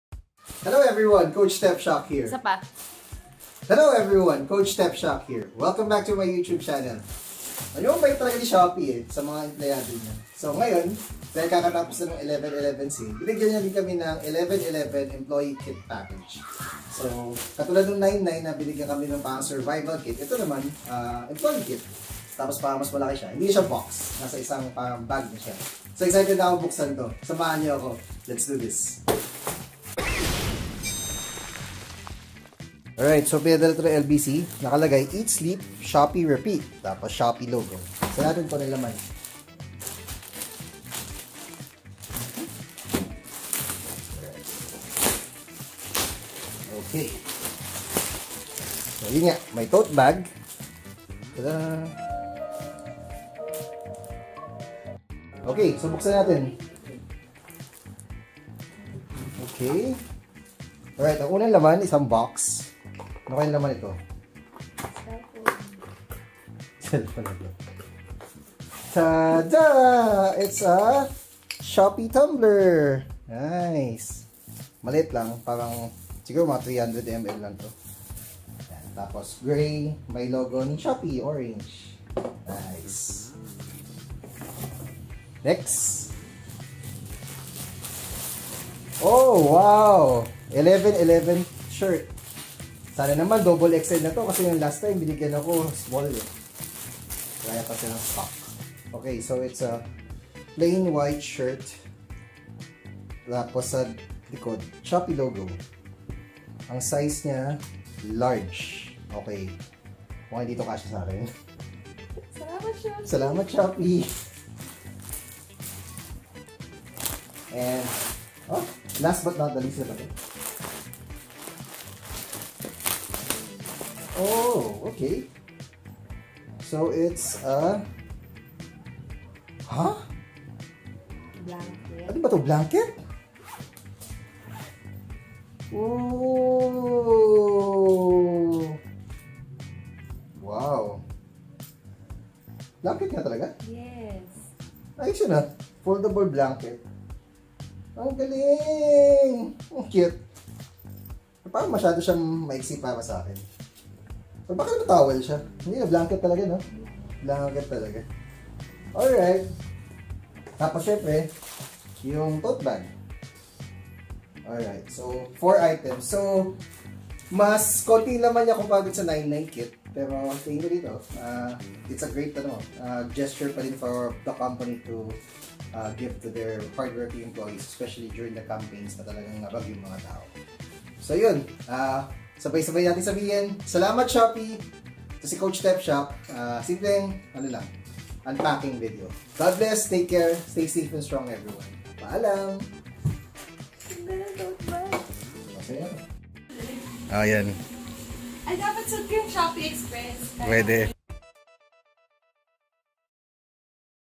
Unpacking the Shopee 11.11 Employee Kit
Isa na namang nakatutuwang pamimigay ni Shopee sa mga empleyado niya.

Katulad noong 9.9 sale campaign, pinadalhan kami ni Shopee ng care kit para sa 11.11 sale campaign.

Samahan ninyo ako sa pag-unpack sa giveaways na ito. Let's do this!